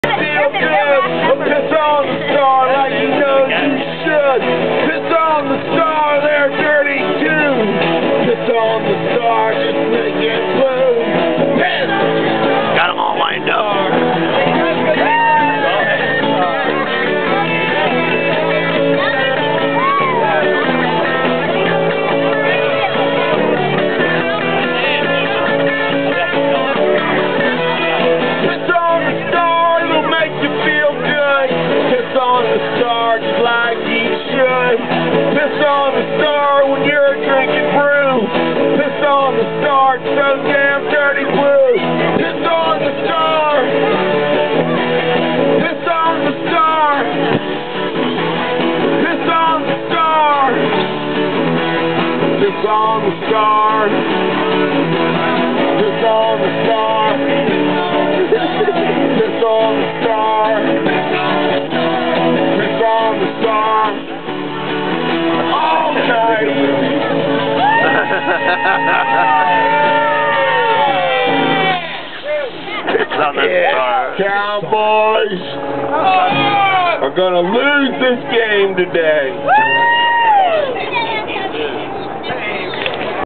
RAC Piss On The Star
The RAC's own Dirtbags advise Redskins fandom young and old to pull their weight in society by Pissing on the Star.